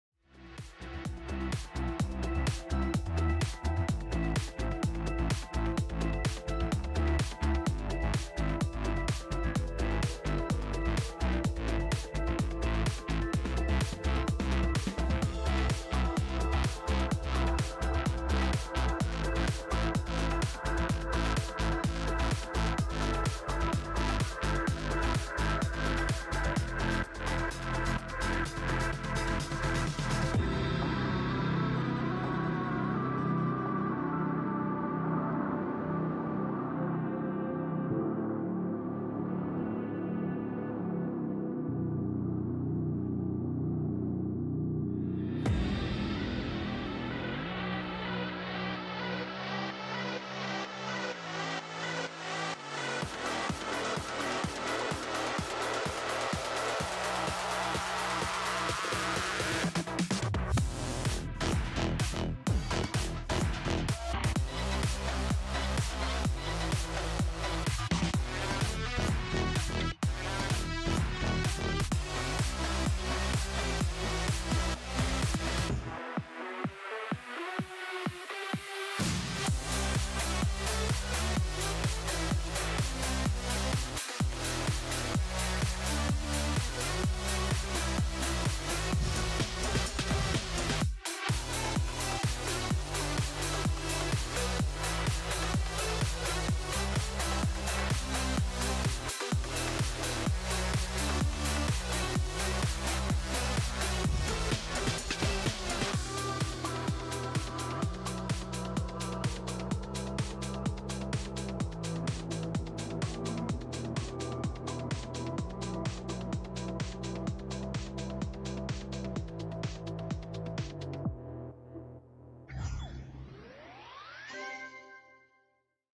"Midnight Moonlight" (Top 1 Layout)
I started this layout in March of 2023. It took so long to complete because I was very specific about what I wanted out of the layout. While I’m very happy with the finished product, I don’t want to spend that much time on a layout ever again. Many hours were sunk into building mechanics that I ended up scrapping because they didn’t fit with what I wanted. It sometimes took weeks to design one single click. I’m happy I pulled through with this, however, and I hope others like it as much as I do.

To be decorated by @zeniuxgd and verified by @hstorm